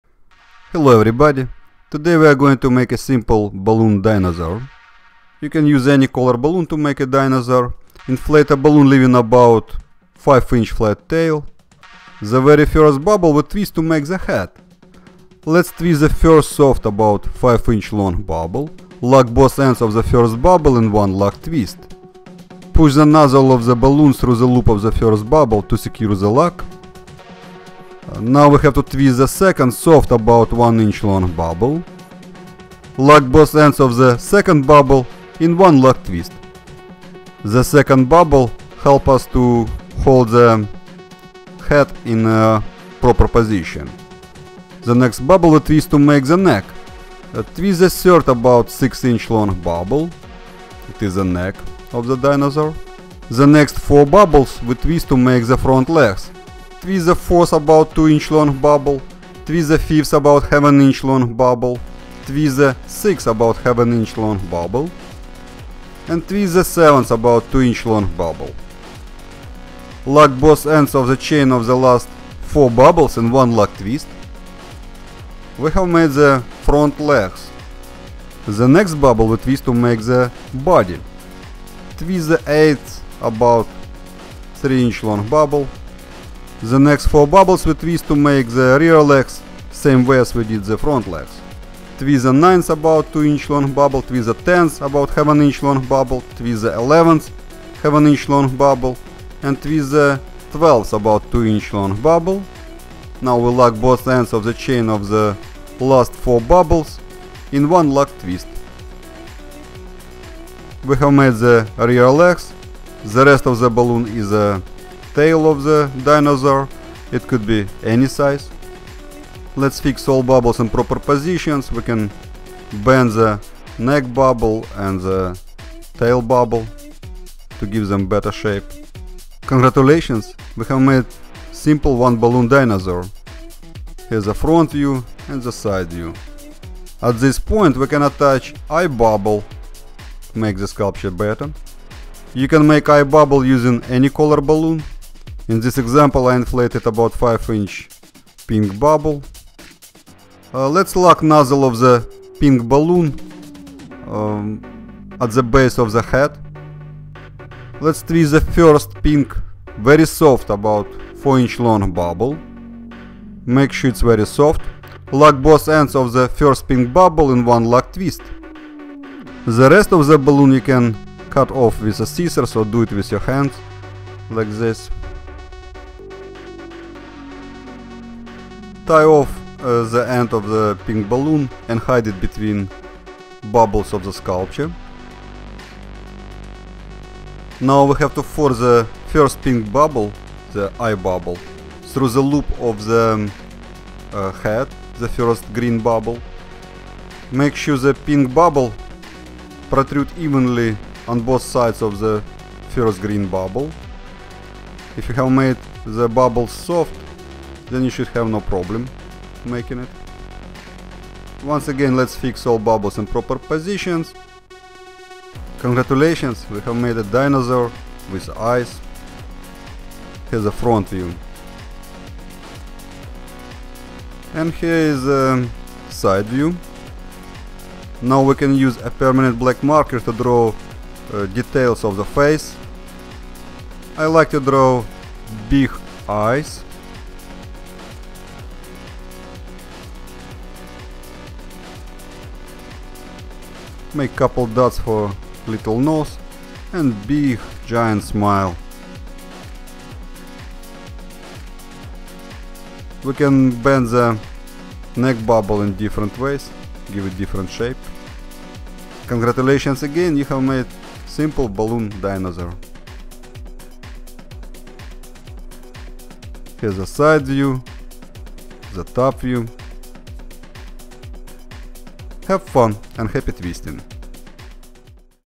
Balloon dinosaur
In this video instruction you will learn how to make balloon dinosaur using balloon twisting technique.

Step by step instructions with photos and text on blog post